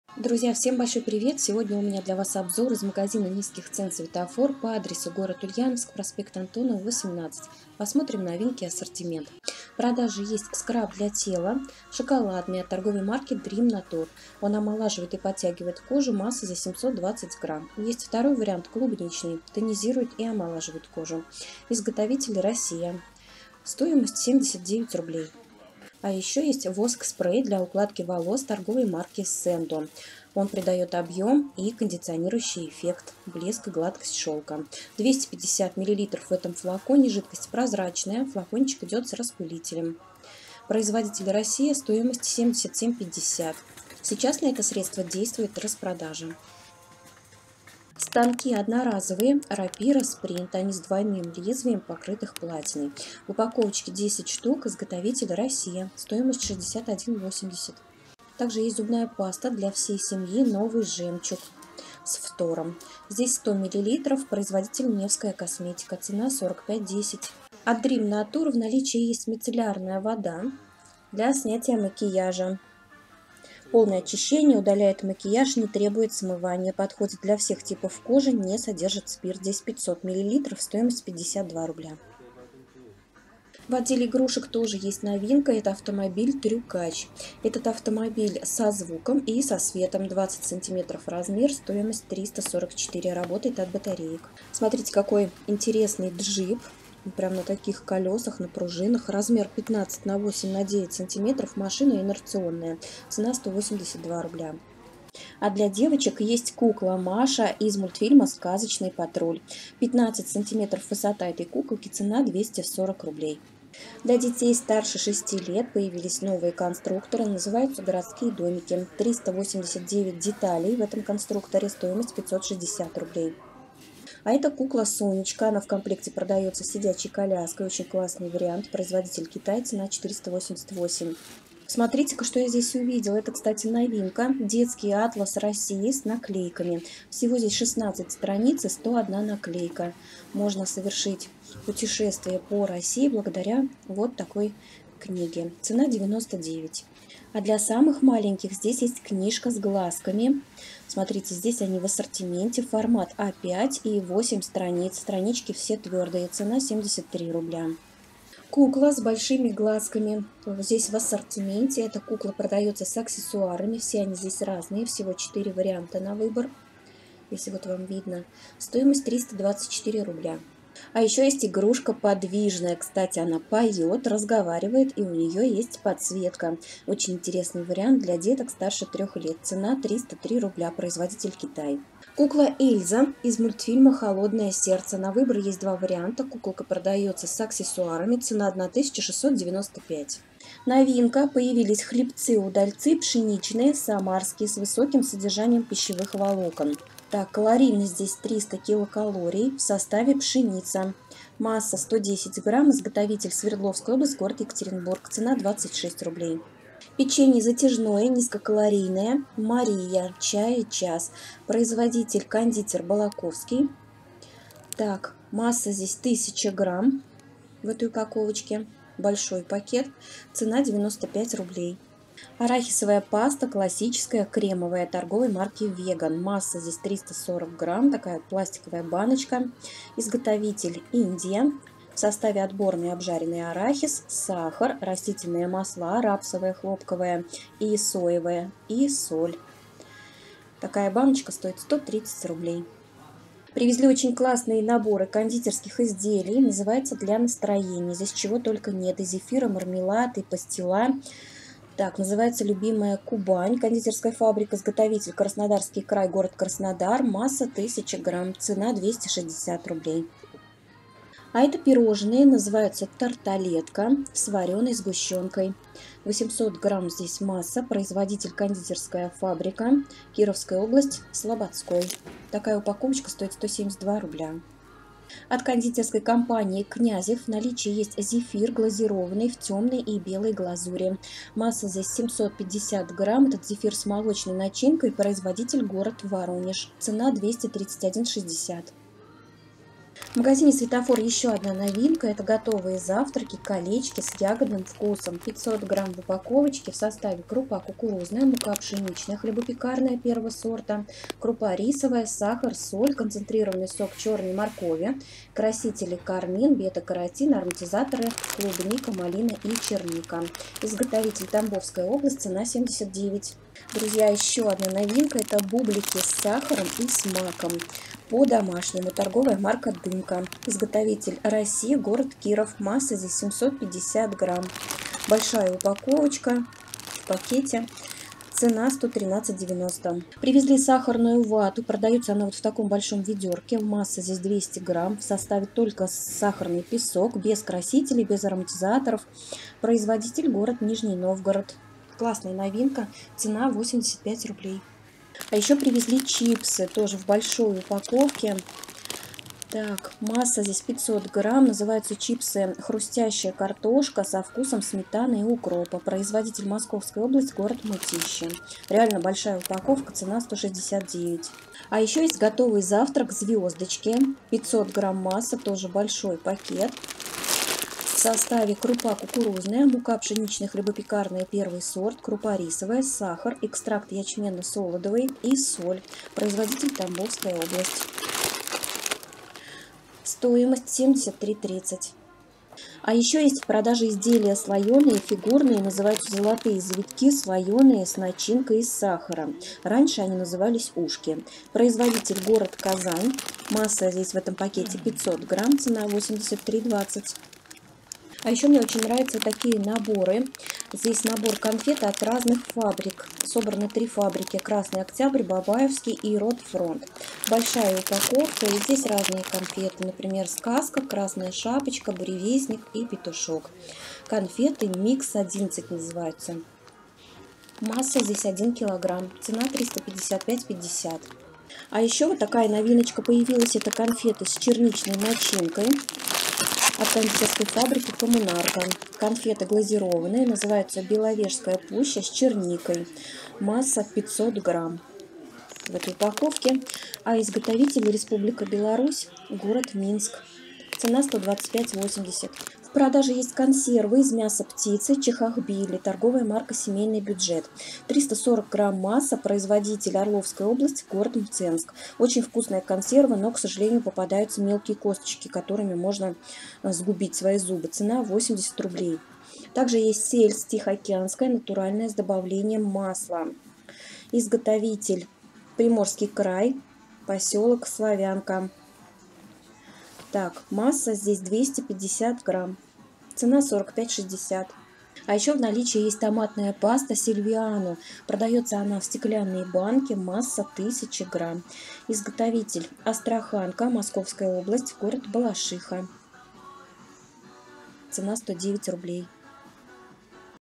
🔥ЗАГЛЯНЕМ В 🚦СВЕТОФОР 🔥 ТУТ ОТЛИЧНЫЙ АССОРТИМЕНТ! МНОГО НОВИНОК ПО НИЗКИМ ЦЕНАМ. ОБЗОР 2023.
Всем привет! Сегодня сделаю обзор новинок из магазина СВЕТОФОР. Новинки СВЕТОФОР 2022. Обзор цен. Видео из магазина низких цен СВЕТОФОР г. Ульяновск проспект Антонова 18.
Присоединяйтесь!